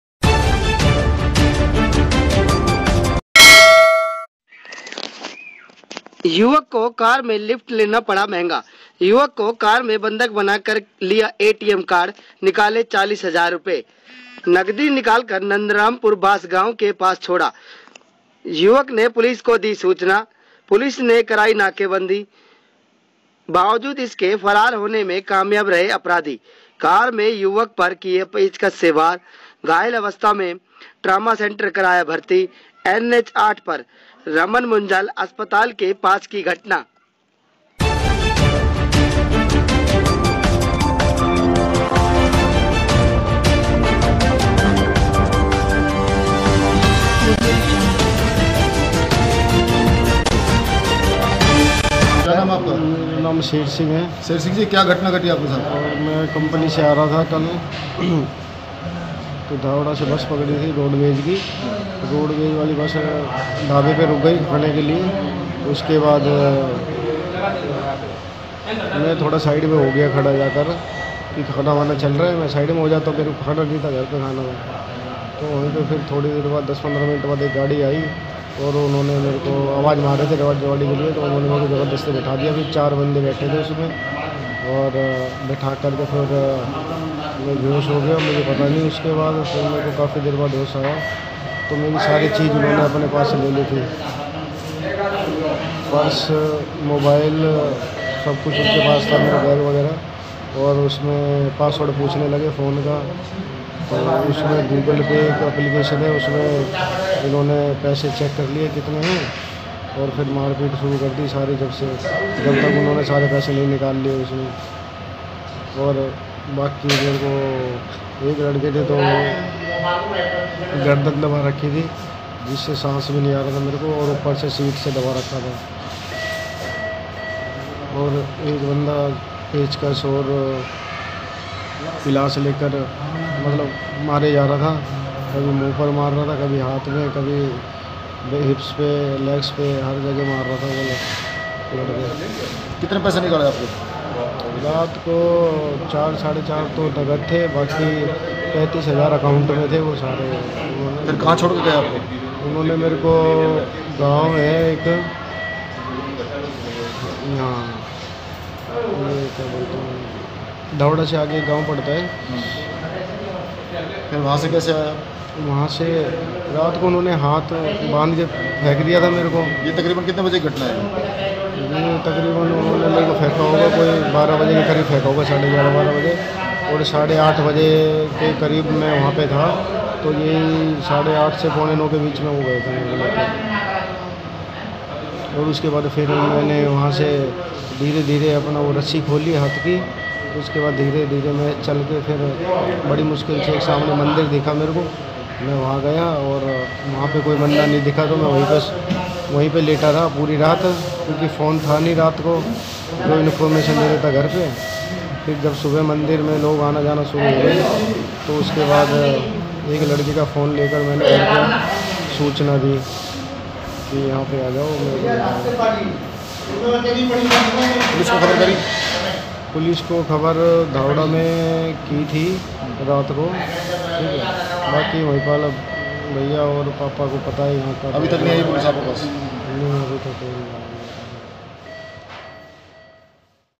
Rewari: कार में लिफ्ट देकर लूटा,,,
चंद्रशेखर व पंकज: रिपोर्टर रेवाड़ी
रेवाड़ी। युवक को कार मे लिफ्ट लेना पड़ा महंगा ,,,
युवक को कार में बंधक बना कर लिया एटीएम,,,
 निकाले 40 हजार रुपए ,,,
नगदी निकालकर नंदरामपुर बास गांव के पास छोड़ा,,,
 युवक ने पुलिस को दी सूचना,,, पुलिस ने कराई नाकेबंदी ,,,
फरार होने में कामयाब रहे अपराधी,,, कार में युवक पर किया पेचकस से वार,,
 घायल अवस्था में ट्रामा सेंटर में कराया भर्ती ,,,
NH8 पर रमन मुंजाल अस्पताल के पास की घटना